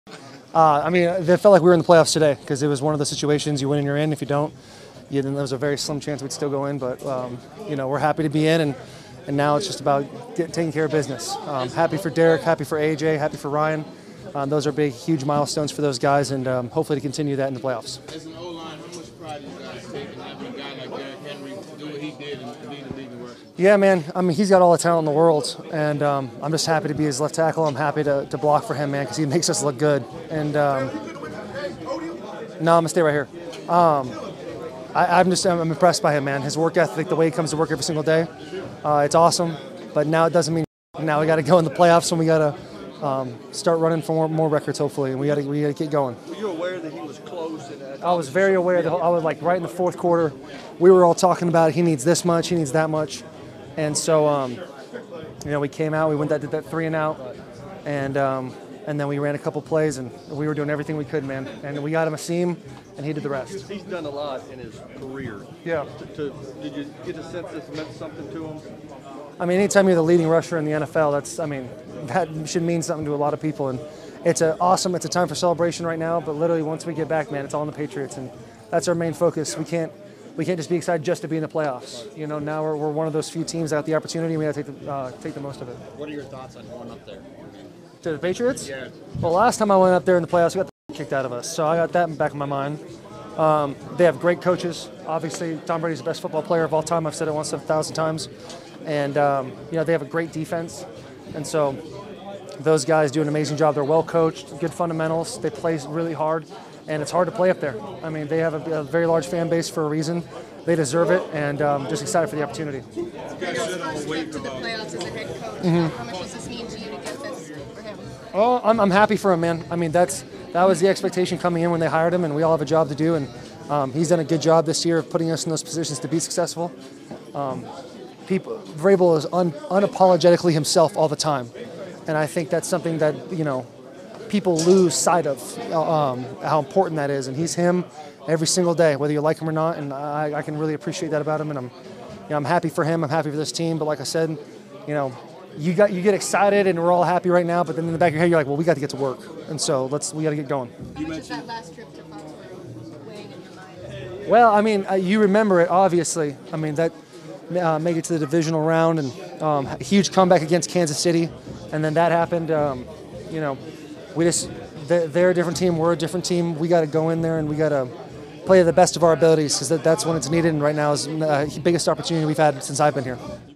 Taylor Lewan: We Can't Be Excited Just to Be in the Playoffs
Titans OT Taylor Lewan speaks with reporters following Sunday’s 35-14 victory over the Houston Texans that punched Tennessee’s ticket to the playoffs.